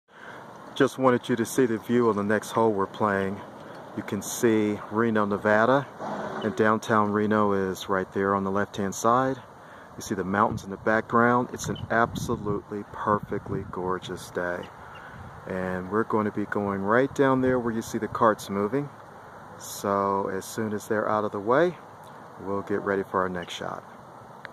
View from hole #15
The view from hole 15 @ Wildcreek Golf Club Reno NV